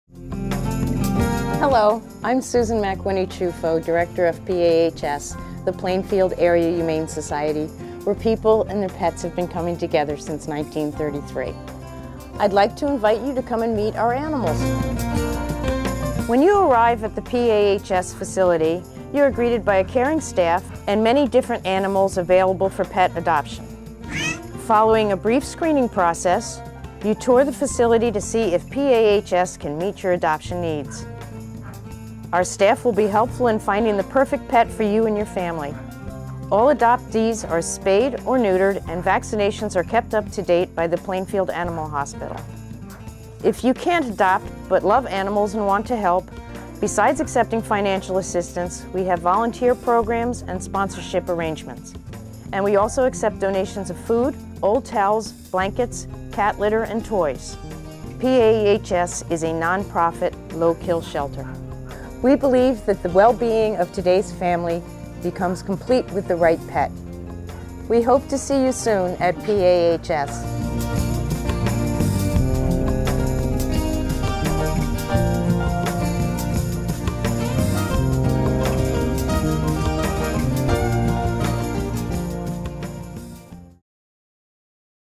Plainfield Area Humane Society video by Jersey Adobe Multimedia, HD version
A video shot and edited by the Jersey Adobe Multimedia (JAM) for the Plainfield Area Humane Society, in NJ.